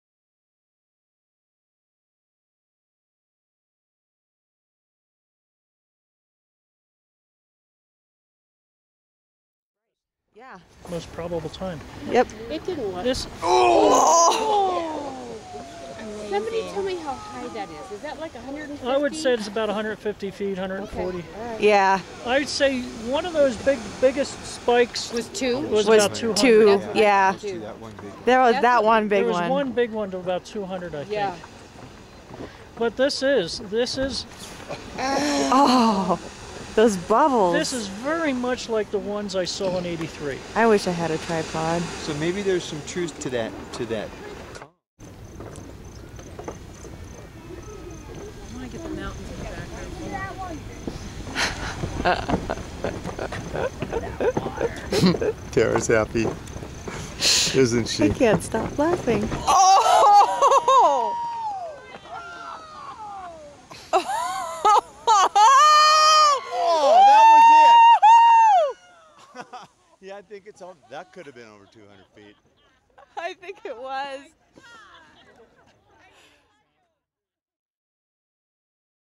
Morning Geyser: June 21, 2012
Excerpts from the June 21, 2012 eruption of Morning geyser.  This rare geyser was last active in April, 1994.